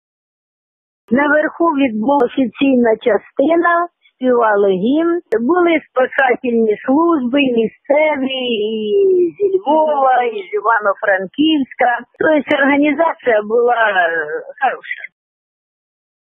2 жителі Полтавщини піднялися на Говерлу
2 жителі Полтавщини взяли участь у 46-ій «Говерліані». Цьогоріч на найвищу точку України піднялися близько 250-ти українців та поляків. Організатори спортивного заходу – учасники львівського альпіністського клубу  «Скеля». Піднялися цього разу на Говерлу професіонали та любителі за 3 години. Про це нашим кореспондентам телефоном розповіла ветеран лижного спорту України Анастасія Дойнікова, яка також піднялася на Говерлу.